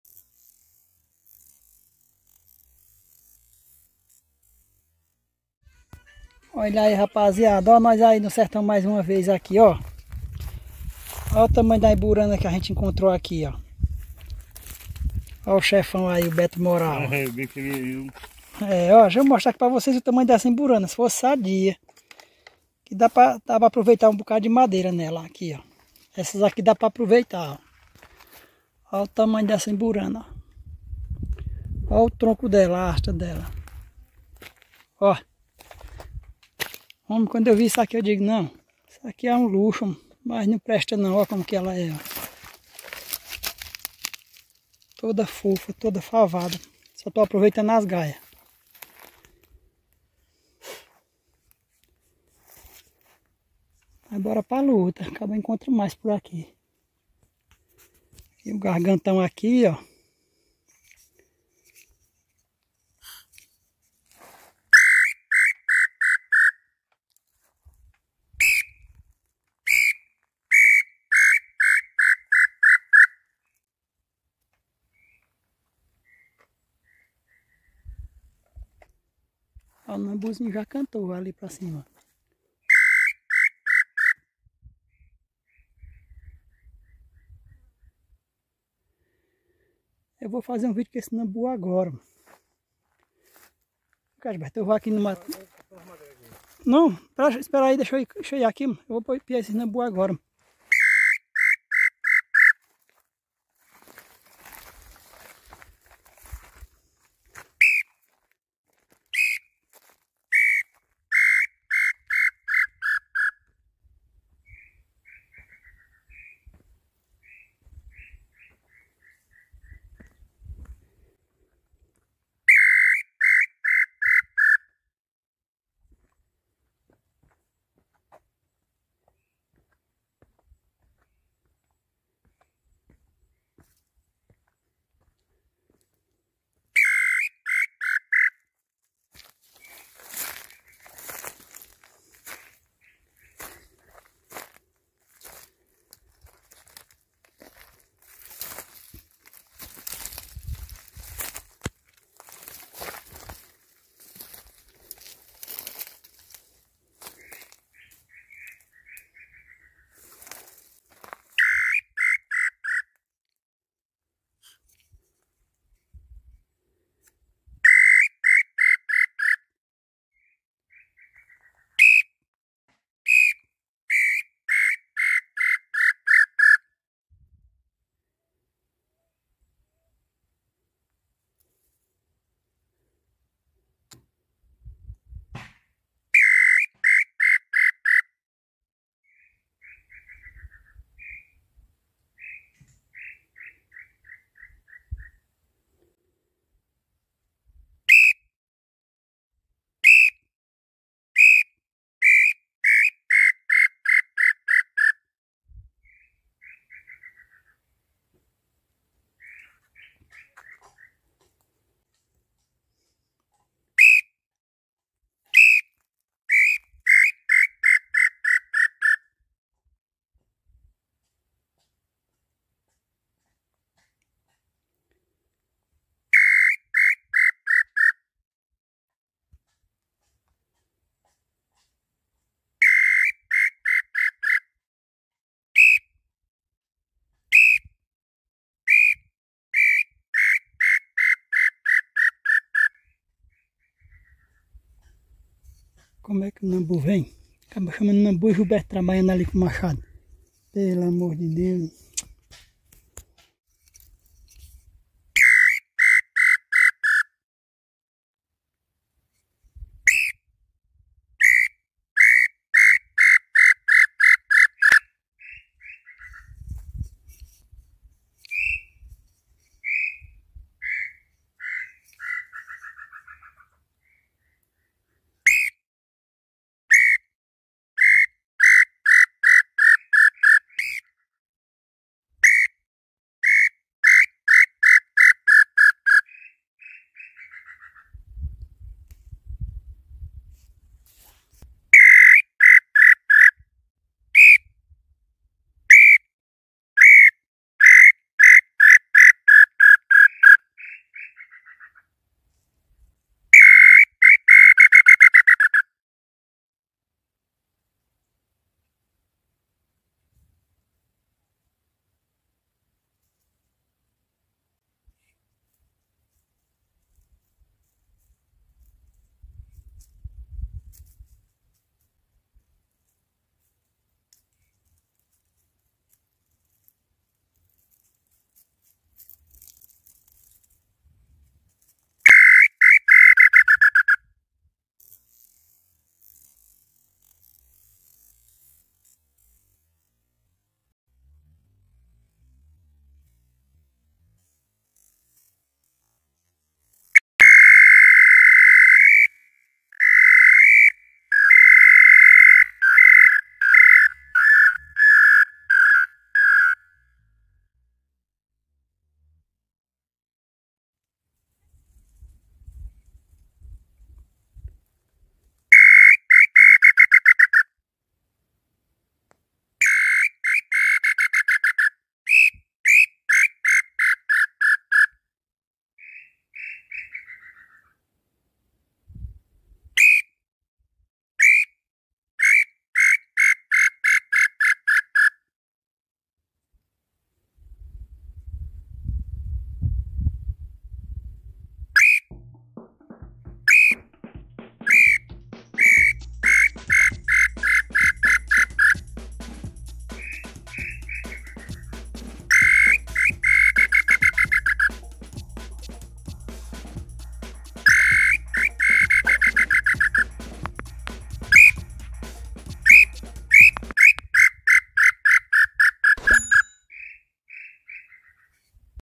LAMBU E UMBURANA NO SERTÃO FERVENDO!!!
Fui na busca da umburana a principal matéria prima do nosso Artesanato os pios. E chamei uma Lambu Pé roxo pra meus pés com o Pio garganta de Nambu pé roxo.

CONTATOS para ADQUIRIR os POIS!